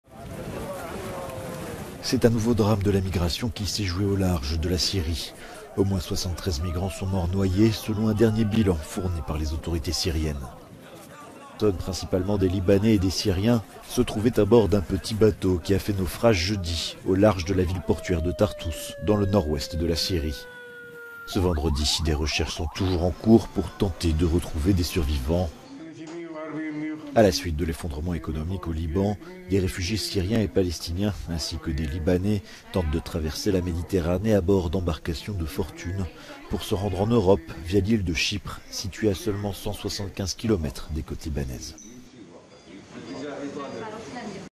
Naufrage d'un bateau au large de la Syrie, des dizaines de morts
Au moins 73 migrants sont morts noyés au large de la Syrie. Le bateau dans lequel ils se trouvaient a fait naufrage. L'embarcation était partie du Liban.